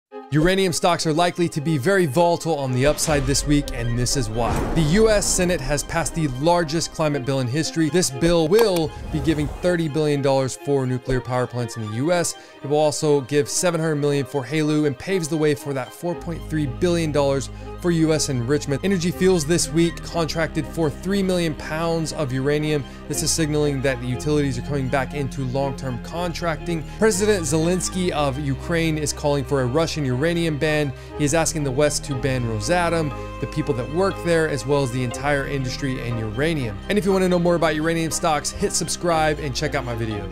Uranium Stocks are going parabolic Now! 💰
Uranium stocks are going to be very volatile this week!
The Senate passed the Largest Energy support bill in history.
This gives 30 billion to U.S. nuclear plants.
700 Million for Haleu.
$150 M for the Office Of Nuclear Energy.
Paves the way for Biden $4.3 billion for U.S. Uranium enrichment.
 $UUUU Reported they added  (3) long-term uranium sales contracts with U.S. nuclear utilities.3.0 million pounds with deliveries to occur during the 2023 – 2030 . Up to 4.3 Mlbs U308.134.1 M wc,$86.4 M cash No debt.
$UEC upped their bid for $UEX over Denison mines.
President Zelensky To the west "Apply tough sanctions against the entire Russian Nuclear industry[Russian state-run nuclear company] Rosatom & all related companies/individuals.”After aiding in Ukraine's reactor takeover.
"Rosatom is at the ZNPP in Ukraine coordinating a special program to cut off ZNPP from the energy system of Ukraine by damaging artillery to the lines. Zelensky calls for World "Sanctions against Rosatom's Nuclear Industry/Individuals."
Thank you for your support for my work!